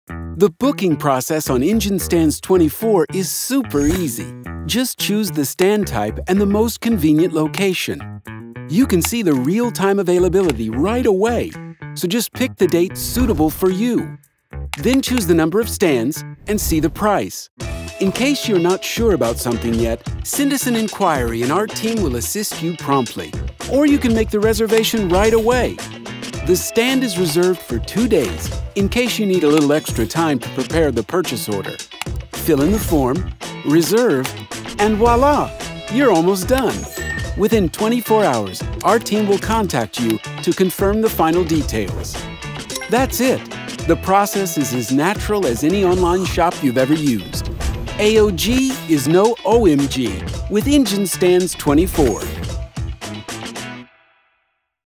EngineStands24.com – quick guide how to book or reserve stand online
Through EngineStands24, you can book an engine stand online without any hassle 24/7!

Check out the video and see how easy and convenient it is to reserve a required stand on our